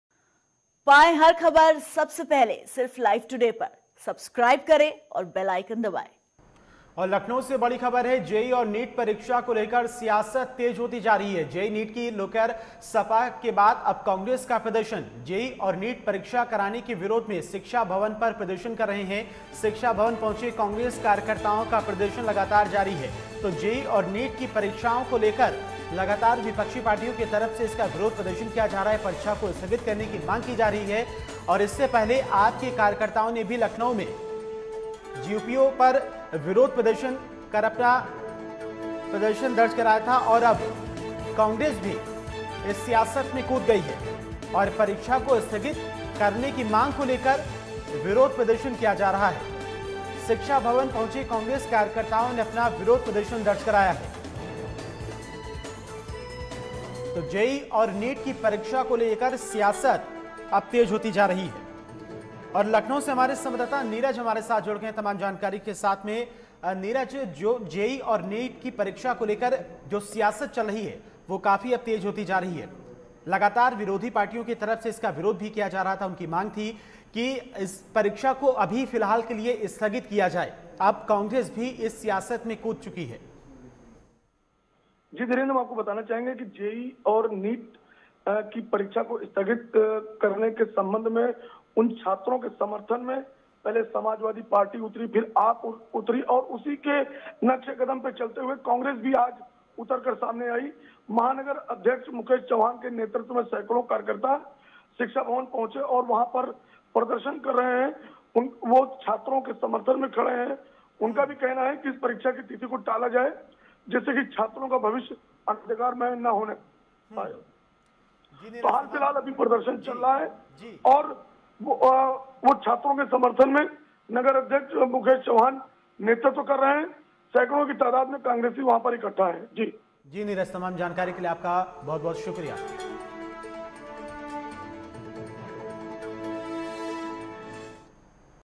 JEE NEET EXAM को लेकर सियासत तेज, शिक्षा भवन पर Congress का प्रदर्शन । Live Today
हमसे जुड़े रहने के लिए हमें फॉलो करें